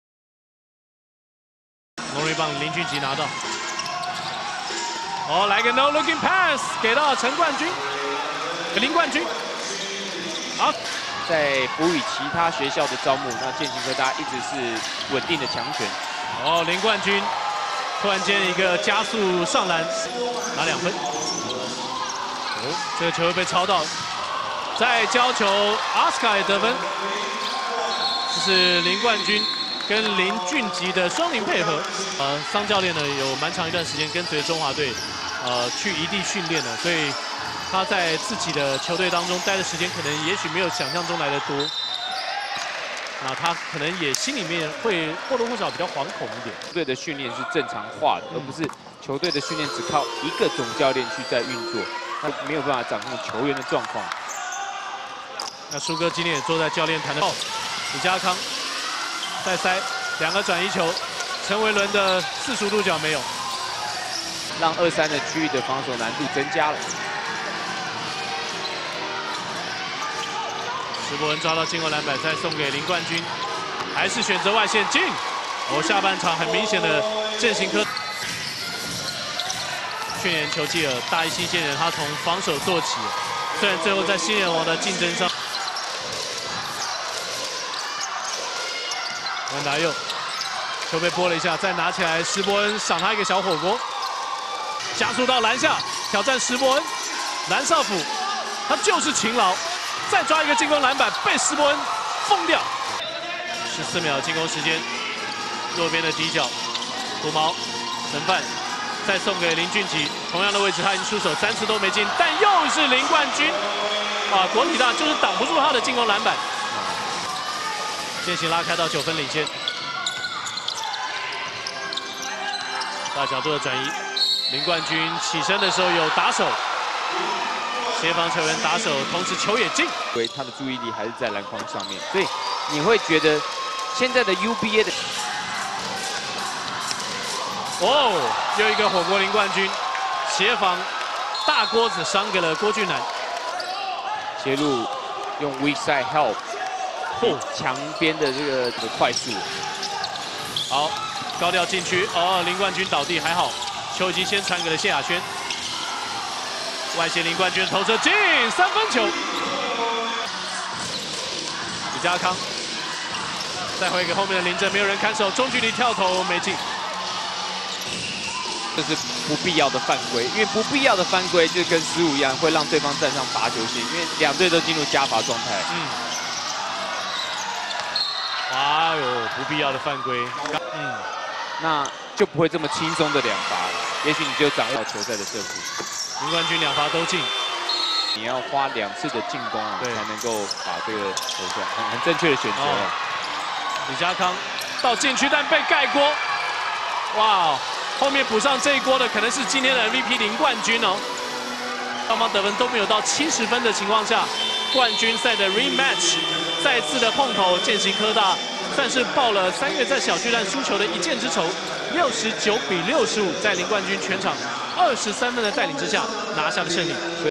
[UBA]2018/11/25 林冠均 23pts 7rb Full Highlights vs 國立體大
影片來源 : SSUTV
視頻都經過編輯，無侵犯版權之意。
如有侵權，將移除內容！
喜歡分享/訂閱鈴鐺按下